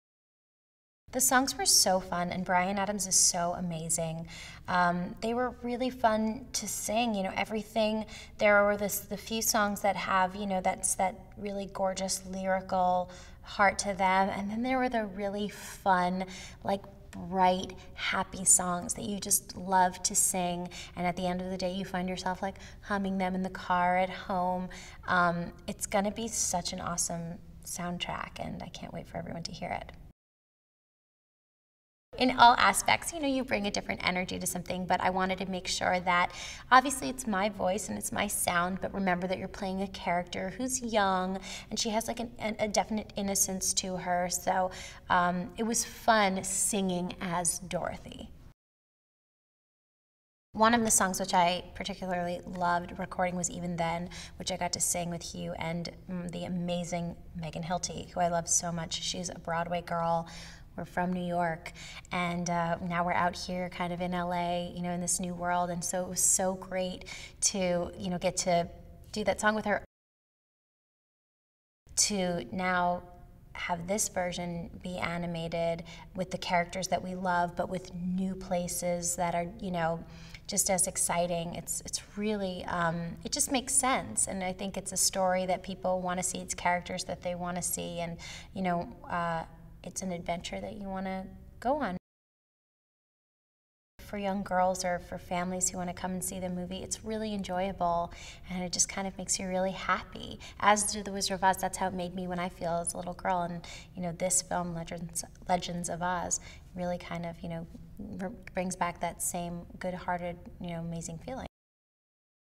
ブライアン・アダムスの曲はおススメよ！映画『オズ めざせ！エメラルドの国へ』リア・ミシェル、インタビュー動画Part2
『プレーンズ』のスタッフと、カナダ出身の人気ミュージシャンのブライアン・アダムスがタッグを組んだアニメーション。ロジャー・スタントン・ボームの児童文学を基に、オズの国に平穏な日々を取り戻そうと奮闘する主人公の活躍を描く。日本語版ボイスキャストに、坂本真綾、三木眞一郎、沢城みゆき、茶風林や人気モデルの香音などが名を連ねる。個性的で楽しいキャラクターたちと共に繰り広げられる、一世一代のアドベンチャーに心奪われる。